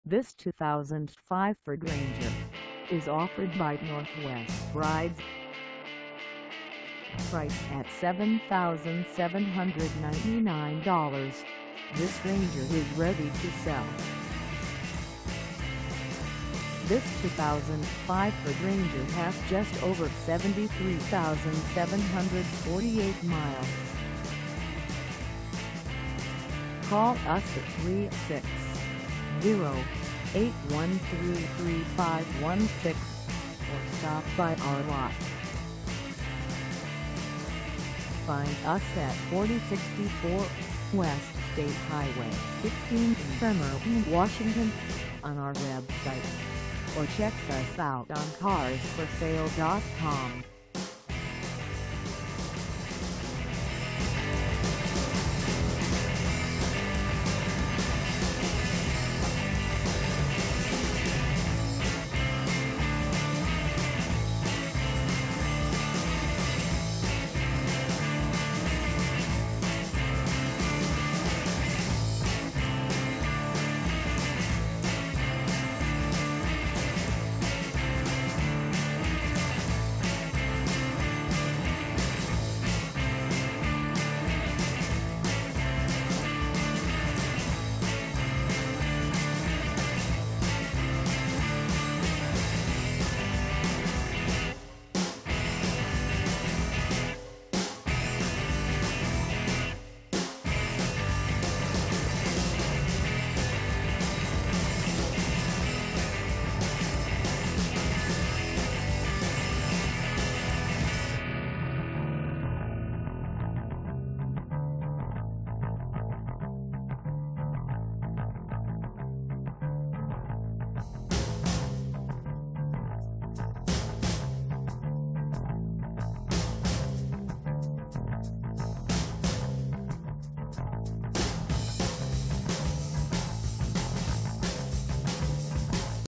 2005 Ford Ranger XL Pickup 2D 6 ft for sale in Bremerton, WA
This 2005 Ford Ranger XL Pickup 2D 6 ft is for sale in Bremerton, WA 98312 at Northwest Rides.

Engine: 2.3L I4
Transmission: Automatic 5-Speed
Color: White
Mileage: 73748
Contact Northwest Rides 4064 W State Hwy 16 Bremerton, WA 98312 to test drive this 2005 Ford Ranger XL Pickup 2D 6 ft today.